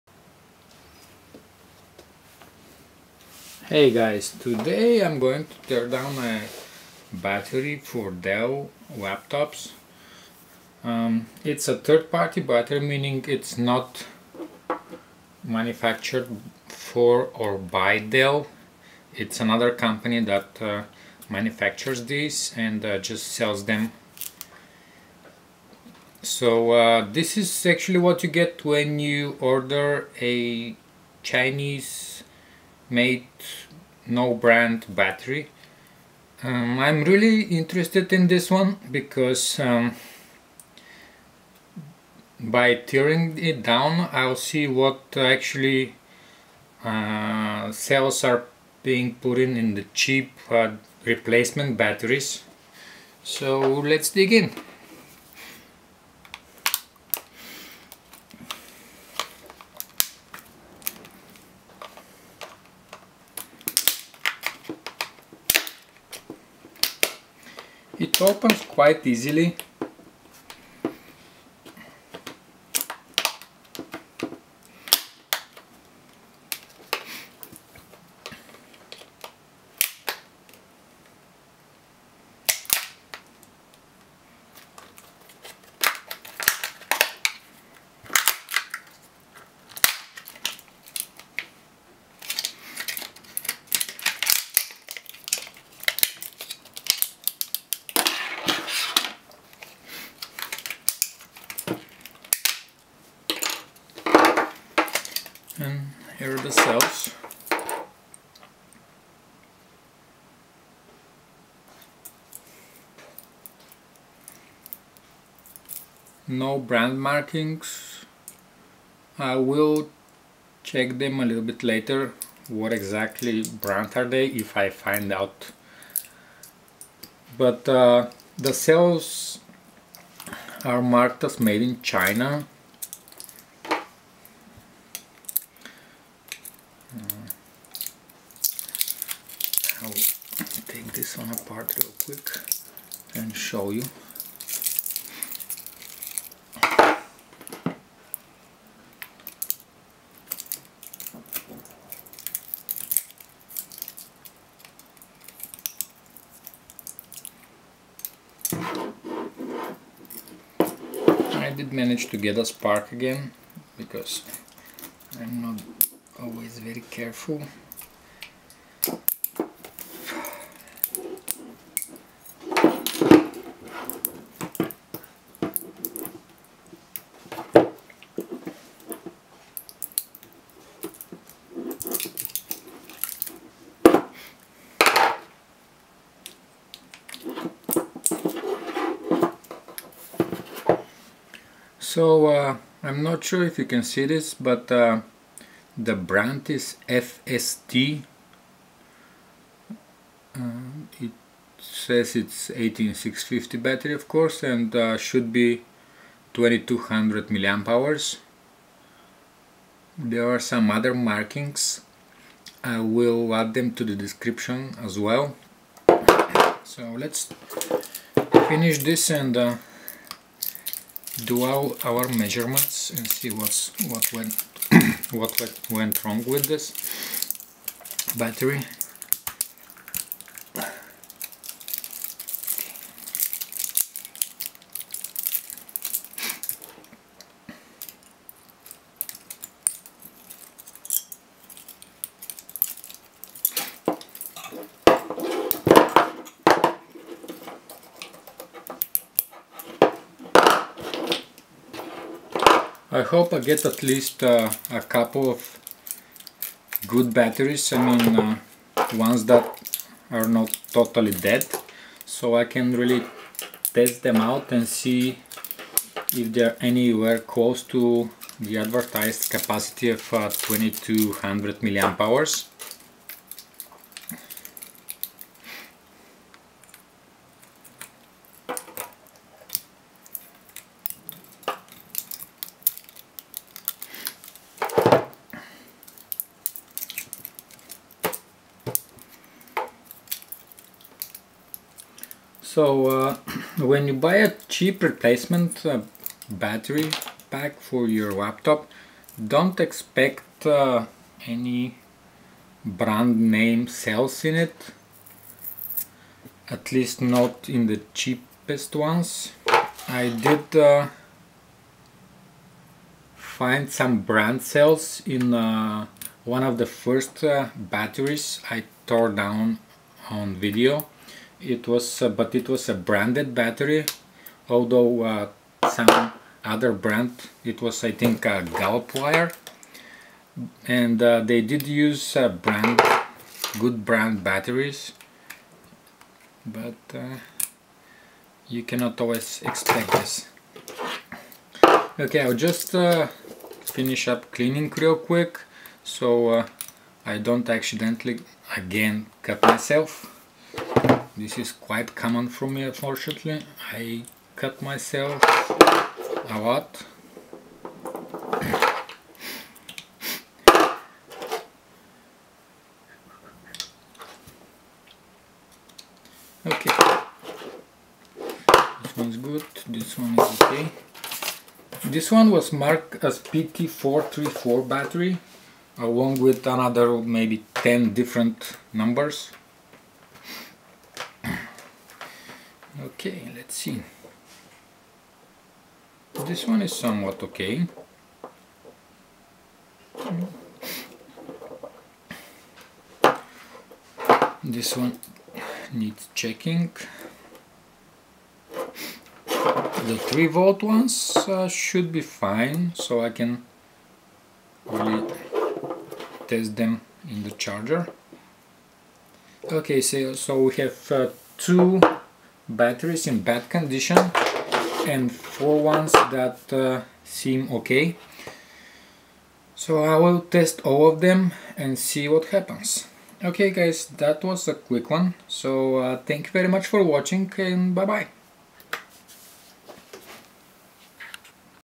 PT434 / FU268 Laptop Battery Teardown - 6x FST 18650 2200 mAh
Teardown of a no-name chinese Dell compatible battery - 11.1V 5200 mAh.
Cells are FST 2200 mAh marked as 7K231S 58332 (made in China).
The pack states to be compatible with Dell part numbers PT434 / PT435 / PT437 / FU268 / FU274 / FU571 / MMP303 / MP307 / NM631 / NM633 / KY477 / KY265 / KY266 / KY268
Battery should be compatible with:
Dell Latitude E6400, E6410, E6500, E6510
Dell Precision M2400, M4400, M4500